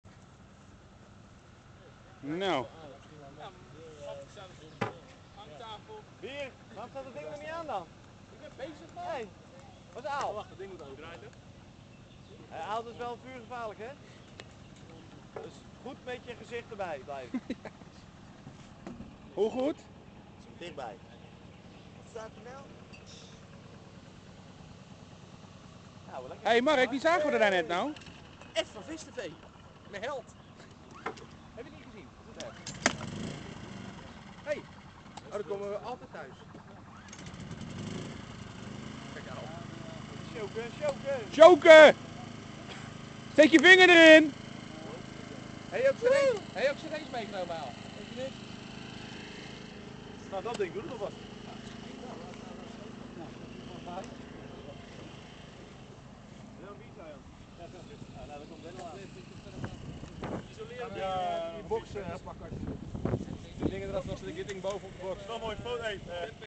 Bachelor Party Ron - The Generator
On the 14th of June, Ron had his Bachelor Party. Here you can see how Alexander and Sebastian are trying to start the generator